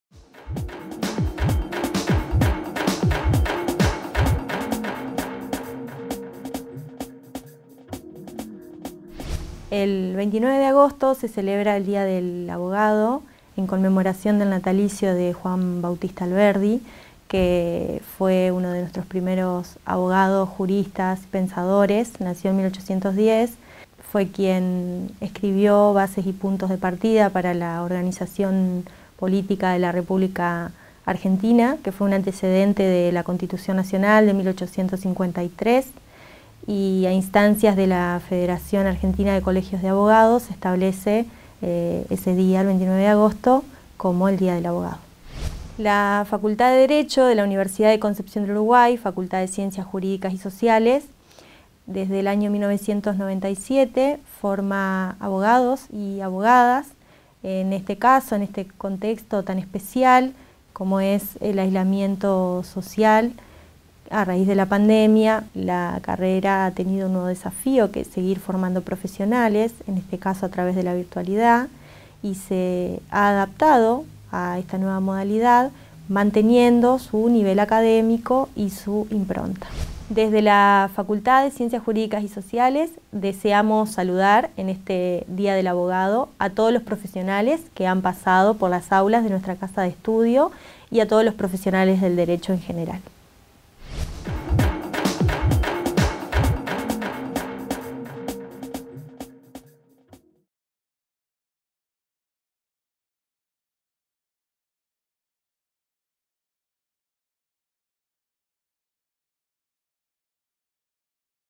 Día del Abogado
Hablamos con la directora de la carrera de Abogacía la  Dra. Romina Sánchez Silveyra  sobre la celebración del 29 de agosto por el día del abogado.